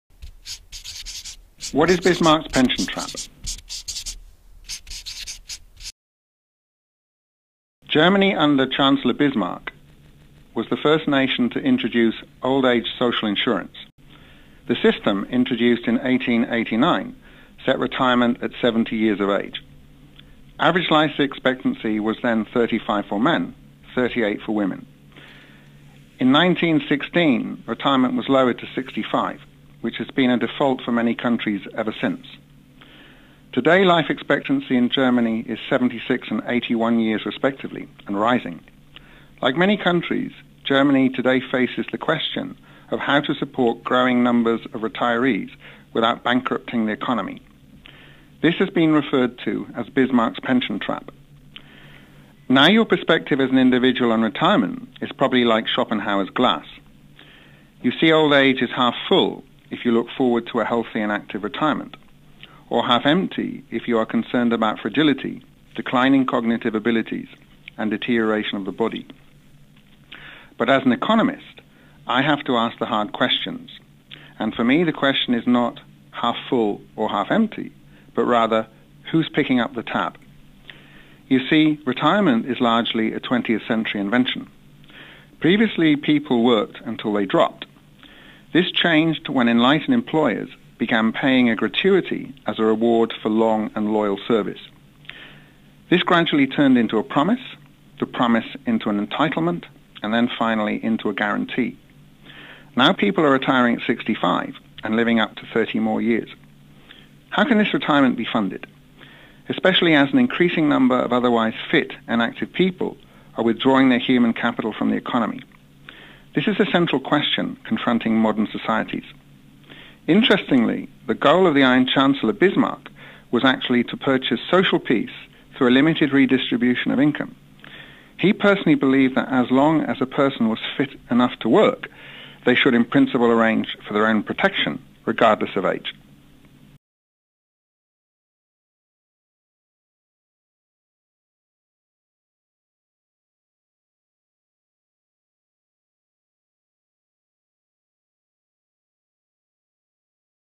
300 Words - Bismarcks Pension Trap
Dr. David Blake is a professor of pension economics at Cass Business School, City of London, and founder and director of the Pensions Institute, a pensions research organization. In 300 words he describes Bismarcks Pension Trap.